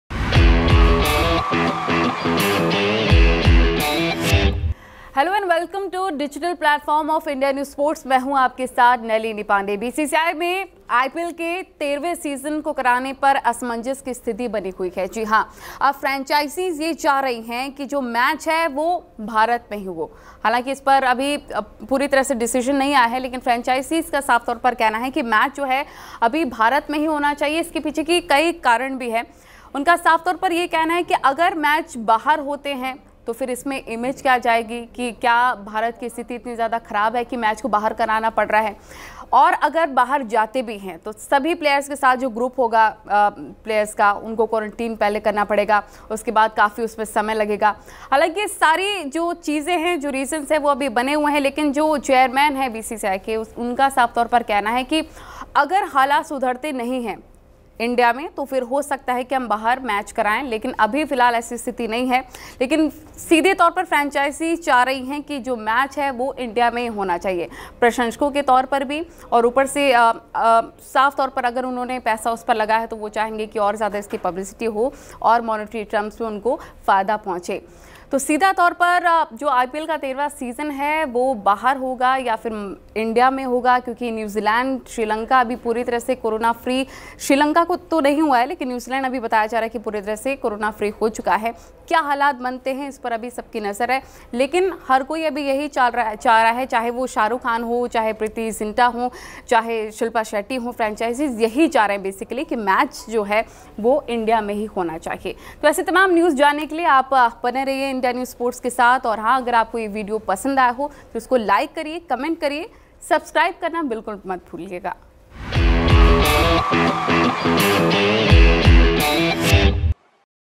Tussle between BCCI & IPL Franchise
Tussle between BCCI and IPL Franchise is going on. BCCI said IPL in foreign soil is the last option but IPL franchise owners are interest only in India